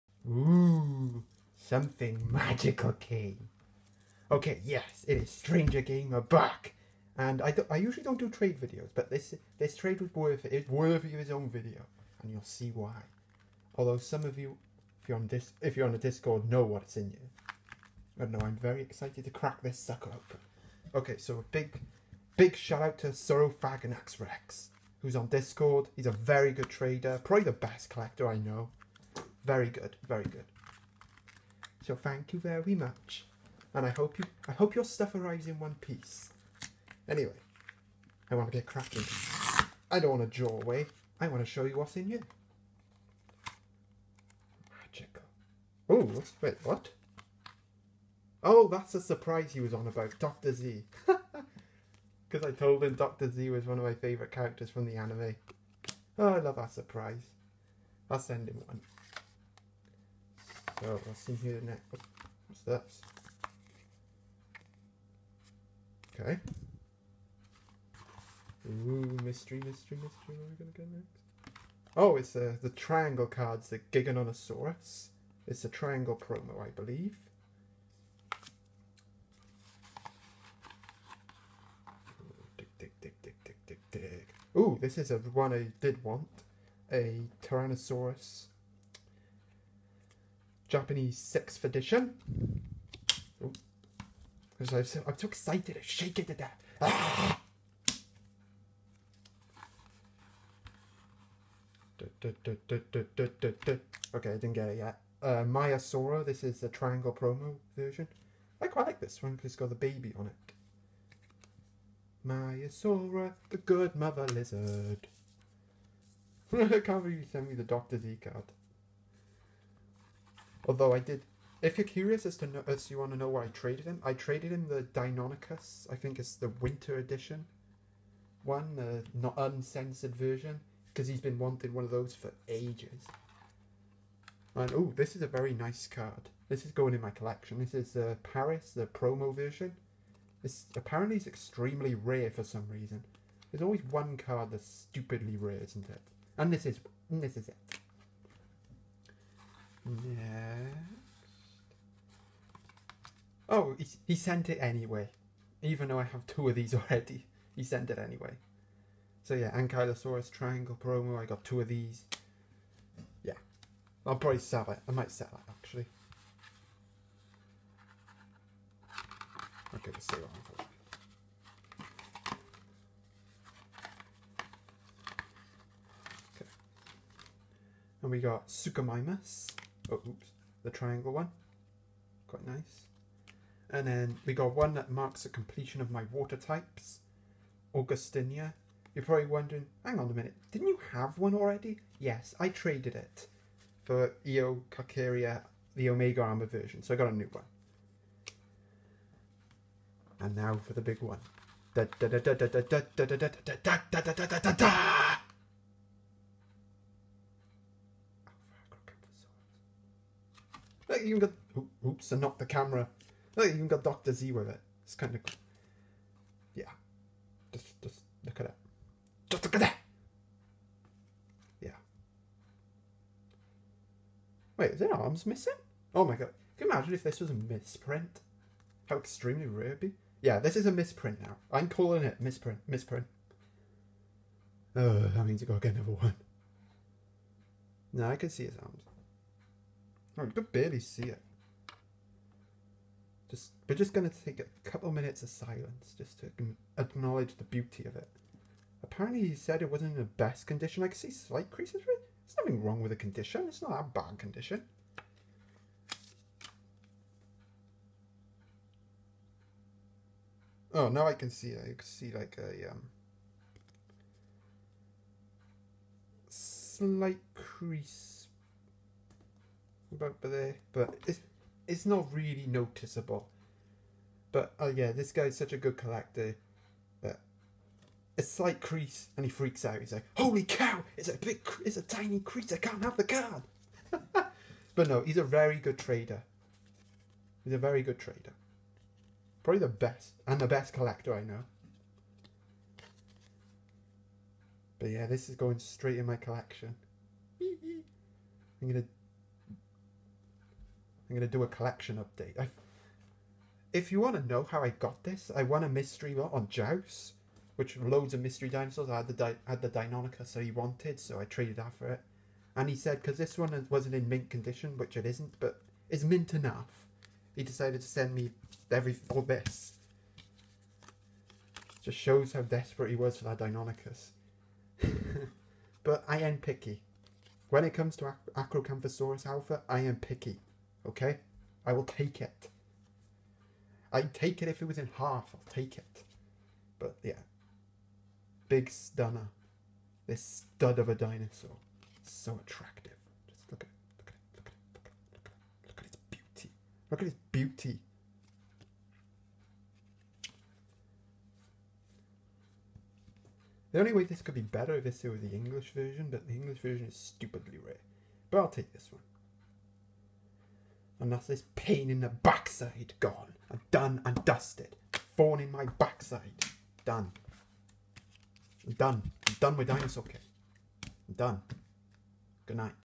Dinosaur king arcade epic trade with Saurophaganaxrex
So while I was busy cleaning this little treasure popped through the letterbox. Its a little out of the blue but well worth it. A big shoutout to Saurophaganaxrex for the trade.